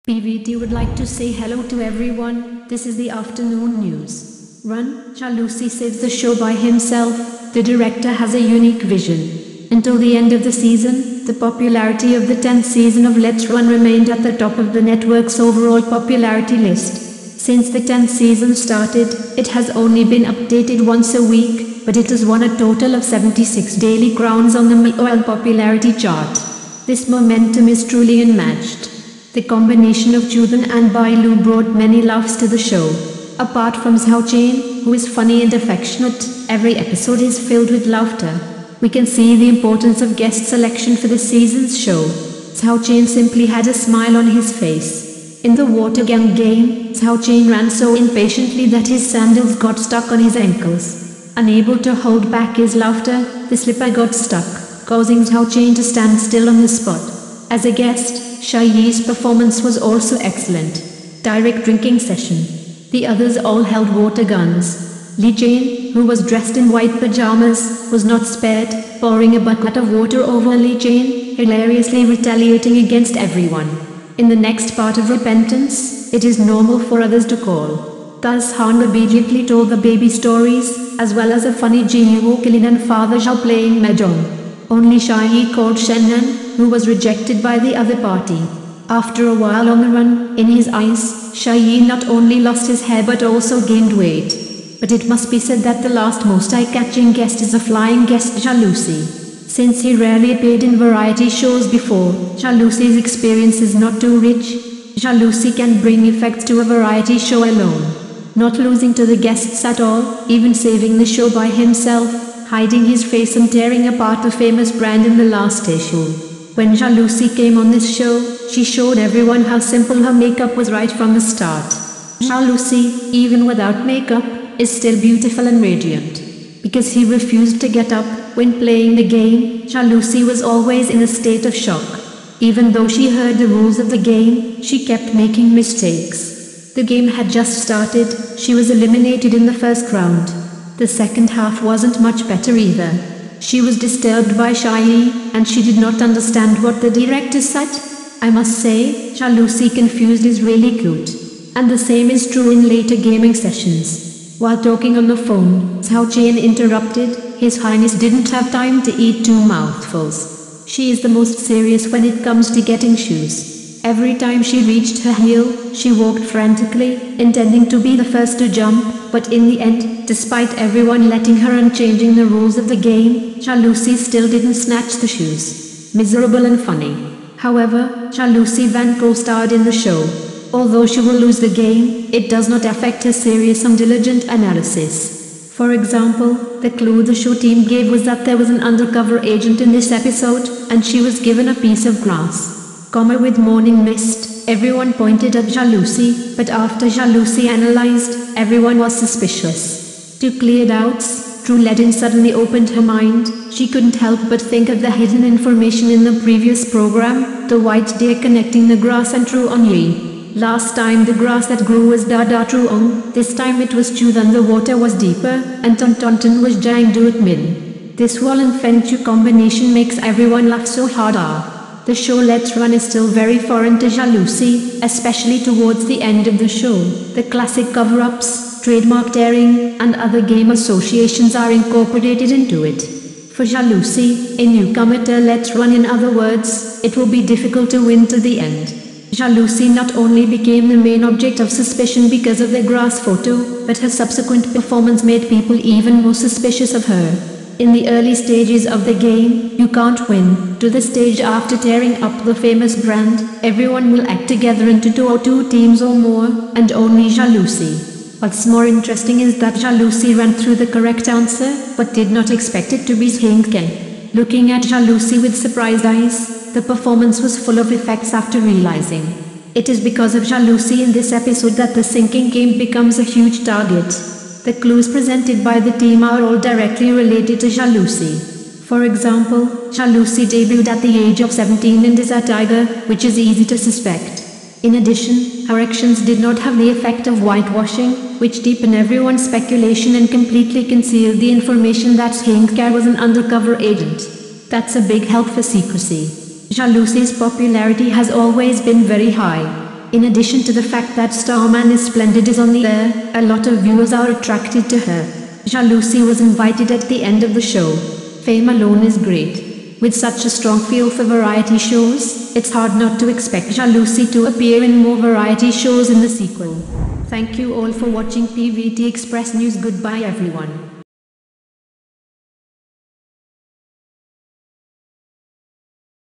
Run: Zhao Lusi saves the show by himself, the director has a unique vision
PVT would like to say hello to everyone, this is the afternoon news.

Until the end of the season, the popularity of the tenth season of "Let's Run" remained at the top of the network's overall popularity list. Since the tenth season started, it has only been updated once a week, but it has won a total of 76 daily crowns on the Maoyan popularity chart. This momentum is truly unmatched.

The combination of Chu Than and Bai Lu brought many laughs to the show. Apart from Zhou Chen, who is funny and affectionate, every episode is filled with laughter.

We can see the importance of guest selection for this season's show. Zhou Chen simply had a smile on his face. In the water gun game, Zhou Chen ran so impatiently that his sandals got stuck on his ankles. Unable to hold back his laughter, the slipper got stuck, causing Zhou Chen to stand still on the spot.

As a guest, Sha Yi's performance was also excellent. Direct drinking session. The others all held water guns. Li Chen, who was dressed in white pajamas, was not spared, pouring a bucket of water over Li Chen, hilariously retaliating against everyone.

In the next part of repentance, it is normal for others to call. Da Zhang obediently told the baby stories, as well as the funny Guo Qilin and father Zhao playing mahjong. Only Sha Yi called Shen Nan, who was rejected by the other party. After a while on the run, in his eyes, Sha Yi not only lost his hair but also gained weight.

But it must be said that the last most eye-catching guest is the flying guest Zhao Lusi. Since he rarely appeared in variety shows before, Zhao Lusi's experience is not too rich. Zhao Lusi can bring effects to a variety show alone. Not losing to the guests at all, even saving the show by himself, hiding his face and tearing apart the famous brand in the last issue.

When Zhao Lusi came on this show, she showed everyone how simple her makeup was right from the start. Zhao Lusi, even without makeup, is still beautiful and radiant.

Because he refused to get up, when playing the game, Zhao Lusi was always in a state of shock. Even though she heard the rules of the game, she kept making mistakes. The game had just started, she was eliminated in the first round. The second half wasn't much better either. She was disturbed by Sha Yi, and she did not understand what the director said?

I must say, Zhao Lusi confused is really cute! And the same is true in later gaming sessions. While talking on the phone, Zhou Chen interrupted, his Highness didn't have time to eat two mouthfuls.

She is the most serious when it comes to getting shoes. Every time she reached her heel, she walked frantically, intending to be the first to jump, but in the end, despite everyone letting her and changing the rules of the game, Zhao Lusi still didn't snatch the shoes. Miserable and funny.

However, Zhao Lusi van co-starred in the show. Although she will lose the game, it does not affect her serious and diligent analysis. For example, the clue the show team gave was that there was an undercover agent in this episode, and she was given a piece of grass. , with morning mist, everyone pointed at Zhao Lusi, but after Zhao Lusi analyzed, everyone was suspicious.

To clear doubts, Trieu Le Dinh suddenly opened her mind, she couldn't help but think of the hidden information in the previous program, the white deer connecting the grass and Truong Ye. Last time the grass that grew was Da Da Truong, this time it was Chu Than the water was deeper, and "Tontonton" was Giang Duat Minh. This "Wolongfengchu" combination makes everyone laugh so hard ah!

The show "Let's Run" is still very foreign to Zhao Lusi, especially towards the end of the show, the classic cover-ups, trademark tearing, and other game associations are incorporated into it. For Zhao Lusi, a newcomer to "Let's Run" In other words, it will be difficult to win to the end.

Zhao Lusi not only became the main object of suspicion because of the grass photo, but her subsequent performance made people even more suspicious of her.

In the early stages of the game, you can't win, to the stage after tearing up the famous brand, everyone will act together into two or two teams or more, and only Zhao Lusi.

What's more interesting is that Zhao Lusi ran through the correct answer, but did not expect it to be Zheng Kai. Looking at Zhao Lusi with surprised eyes, the performance was full of effects after realizing.